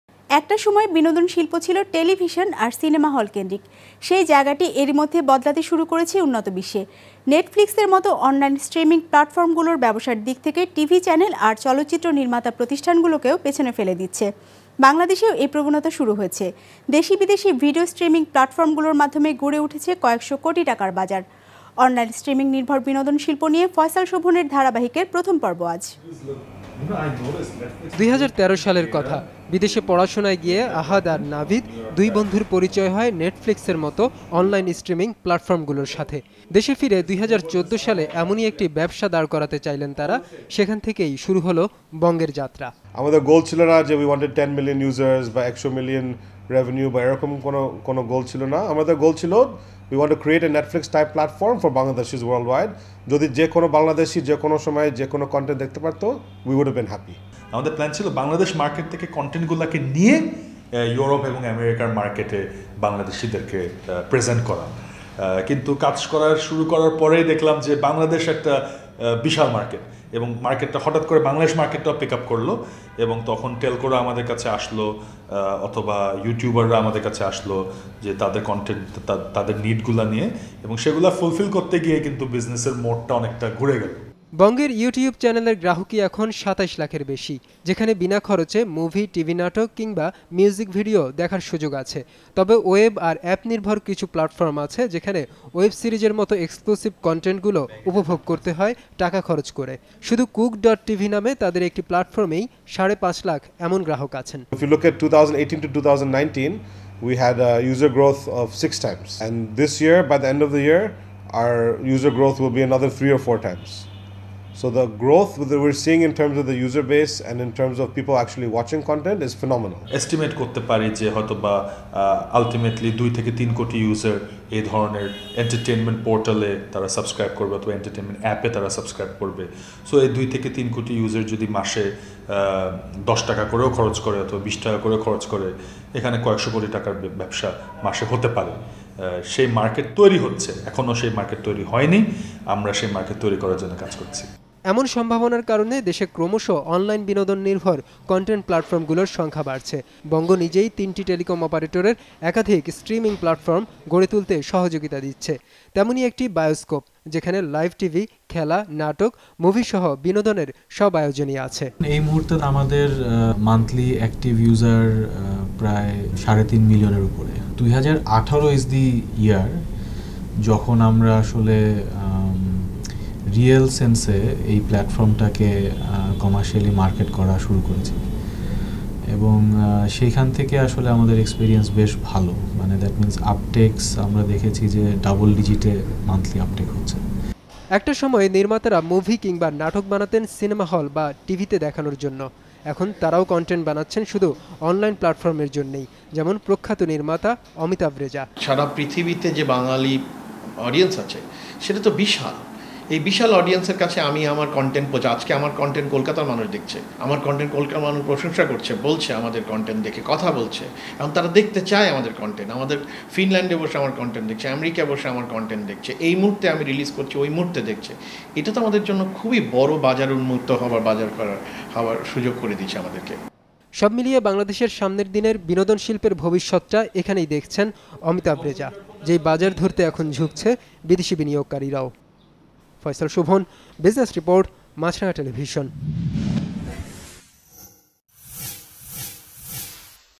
বিনোদন ঝুঁকছে অনলাইন স্ট্রিমিং-এ...
একটা সময়ে বিনোদন শিল্প ছিল টেলিভিশন আর সিনেমা হল কেন্দ্রিক। সেই জায়গাটি অনেকটাই বদলাতে শুরু করেছে উন্নত বিশ্বে। নেটফ্লিক্সের মত অনলাইন স্ট্রিমিং প্ল্যাটফর্মগুলো ব্যবসার দিক থেকে টিভি চ্যানেল আর চলচ্চিত্র নির্মাতা প্রতিষ্ঠানগুলোকও পেছনে ফেলে দিচ্ছে। বাংলাদেশেও এই প্রবণতা শুরু হয়েছে। দেশী বিদেশী ভিডিও স্ট্রিমিং প্লাটফর্মগুলোর মাধ্যমে গড়ে উঠছে কয়েকশো কোটি টাকার বিনোদন বাজার। বাংলাদেশের অনলাইন স্ট্রিমিং নির্ভর বিনোদন শিল্পের খবর জানা যাক এবার...